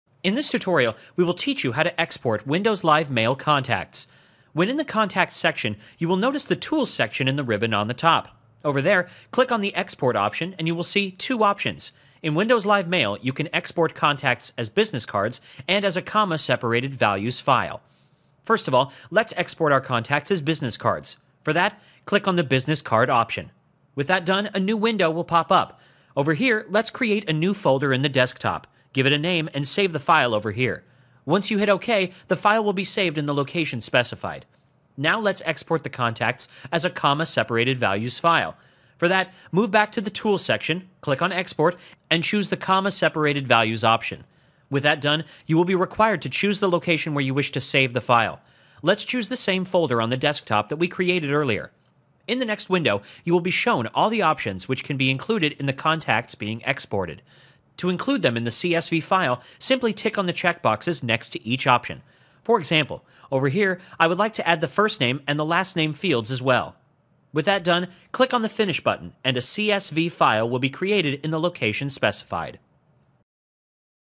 How to Export Windows Live Mail Contacts in Windows 7
Learn how to export contacts in windows live mail. Learn how to export them in the form of business cards and also in the form of a CSV file.

In this tutorial, we will teach you how to export windows live mail contacts. There are two ways in which contacts can be exported in windows live mail. We will cover both the options in detail in this step by step guide.

Step 1 -- Click on Export

When in the contacts section, you will notice the tools section in the ribbon on the top. Over there, click on the export option and you will see two options. In windows live mail, you can export contacts as business cards or in a comma separated values file.
First of all, let's export our contacts in windows live mail as business cards. For that, click on the business card option.

Step 2 -- Set destination folder

With that done, a new window will pop up. Over there, let's create a new folder on the desktop. Give it a name and save the file over there. Once you hit the ok button, the file will be saved in the location specified.

Step 3 -- Comma separated values

Now let's export the contacts in windows live mail in the form of a comma separated values file. In order to do that, go back to the tools section, click on export and choose the comma separated values option.

Step 4 -- Set Destination Path

With that done, you will be required to choose the location where you wish to save the file. Let's choose the same folder on the desktop that we created earlier.

Step 5 -- Choose fields you want to export

In the next window, you will be shown all the options which can be included in the contacts being exported. To include them in the csv file, simply tick the checkboxes next to each option. For example, in this tutorial, we would like to add the first name and the last name fields both.
With that done, click on the finish button and a csv file will be created in the location specified. Hope you found this how to export windows live mail contacts guide useful.